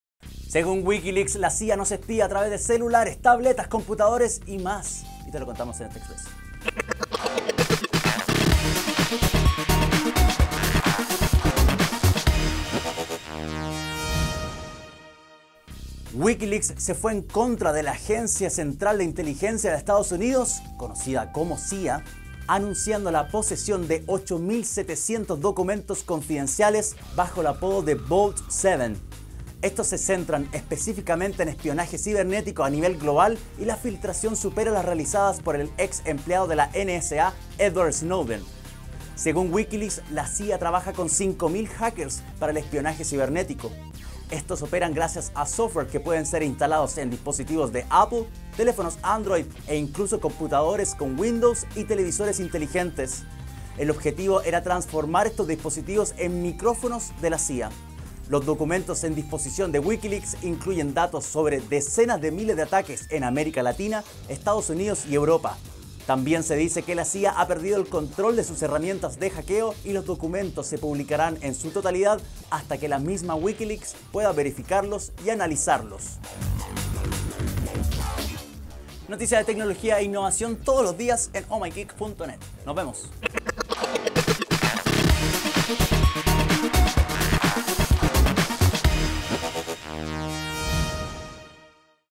OMG! Express: Wikileaks cuenta cómo la CIA nos espía (Vault 7)
'Vault 7' es la filtración más grande que Wikileaks haya hecho respecto a documentos clasificados de la Agencia Central de Inteligencia (CIA) de Estados Unidos en la historia. En EXPRESS te contamos los detalles preliminares.